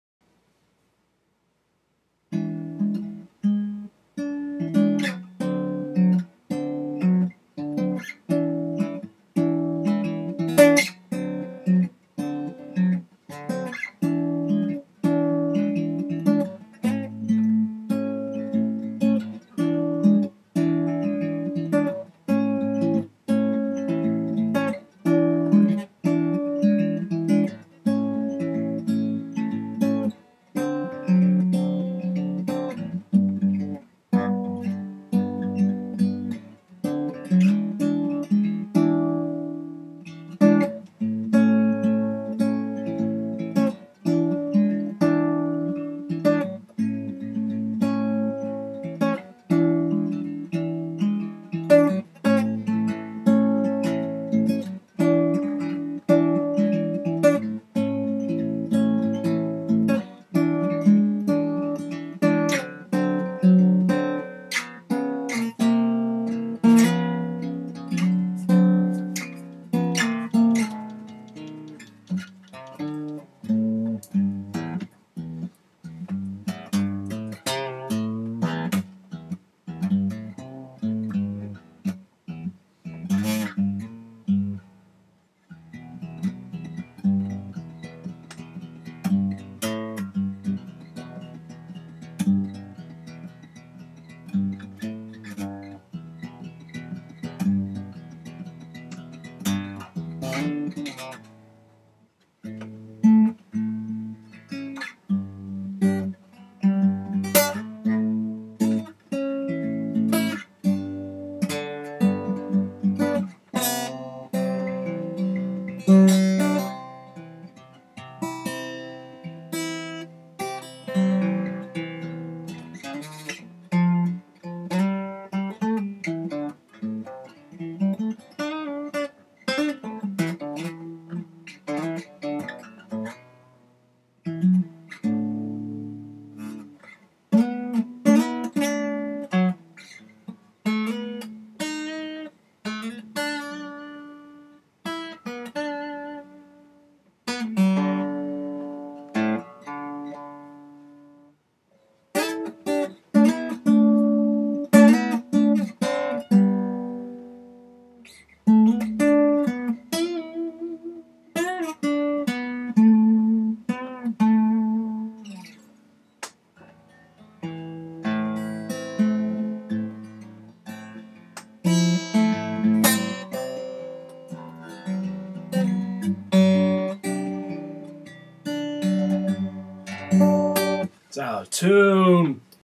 screwing around on guitar
this video is 100% my style, excluding the pink floyd solo half way through. So if you don't like my style, you can f off. The strings are brand new, hence the slight out of tuneness towards the end